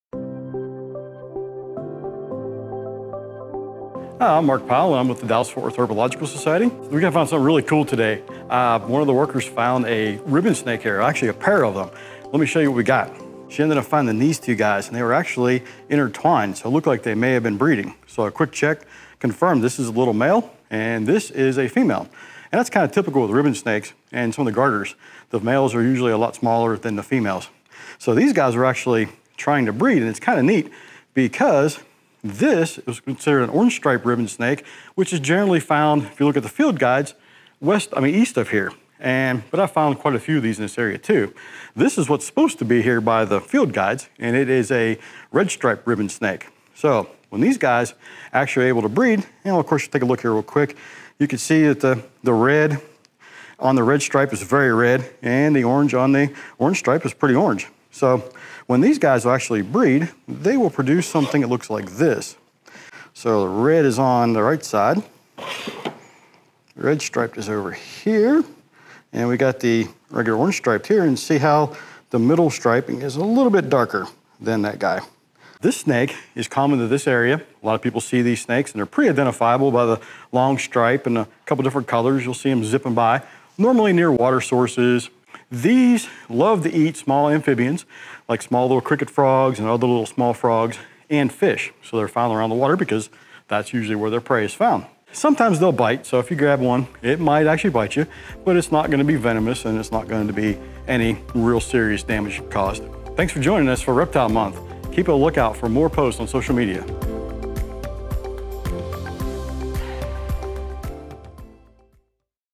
Western Ribbonsnake | Fossil Rim Wildlife Center
Mark Pyle is back with Fossil Rim for more reptile month awesome-ness. This week he has a non-venomous snake that if you are near us here at FR then you will most likely see in your own backyard.